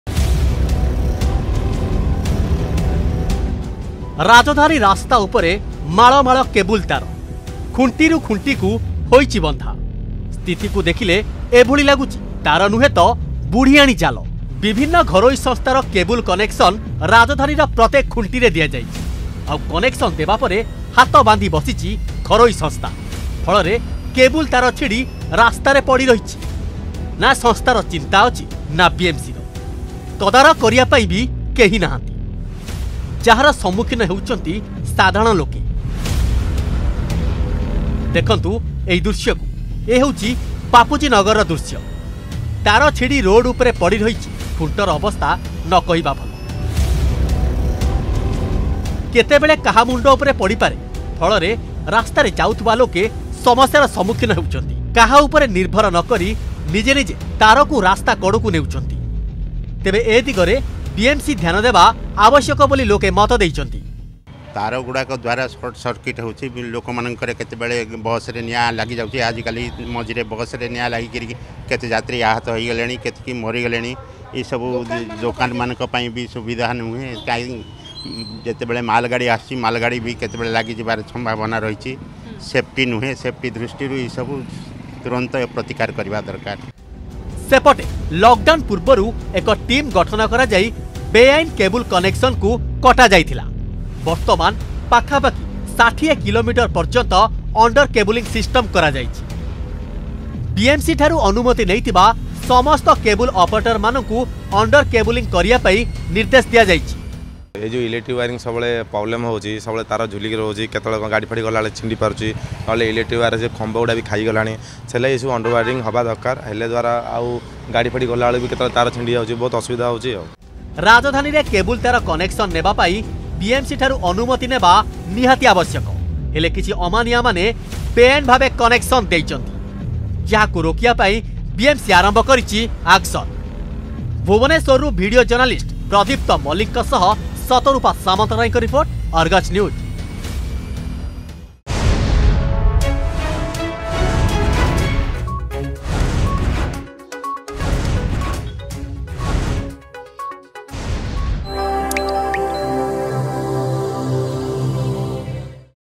ରାଜଧାନୀ ରାଜରାସ୍ତାରେ ପଡ଼ିରହିଛି ପୁଳା ପୁଳା କେବୁଲ ତାର...
ରାଜଧାନୀ ରାଜରାସ୍ତାରେ ପଡ଼ିରହିଛି ପୁଳା ପୁଳା କେବୁଲ ତାର । ସହରର ପ୍ରତ୍ୟୋକ ଜାଗାରେ ଦେଖିବାକୁ ମିଳିଛି ଏଭଳି ଦୃଶ୍ୟ । ନା କାହାର ଚିନ୍ତା ଅଛି ନା ଚେତନା । ଘରୋଇ ସଂସ୍ଥା କେବୁଲ କନେକସନ ଦେବା ପରେ ହାତବାନ୍ଧି ବସୁଛନ୍ତି । ଫଳରେ ତାର ଛିଡି ଦୁର୍ଘଟଣାର ସମ୍ମୁଖୀନ ହେବା ସହ ବିଭିନ୍ନ ସମସ୍ୟା ଭୋଗୁଛନ୍ତି ରାଜଧାନୀ ବାସିନ୍ଦା.